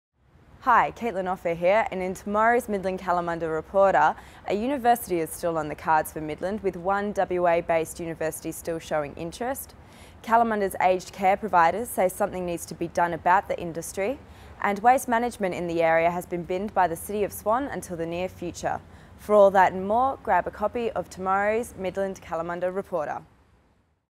Midland / Kalamunda Reporter 15/08/111
Kaitlyn Offer with some of the main stories to appear in tomorrow's Midland / Kalamunda Reporter (15-08-11)

Footage shot by CNG photographer, Matthew Poon
Canon 1D Mk4
Canon 70-200mm f/2.8L IS USM II
Audio-Technica AT803 mic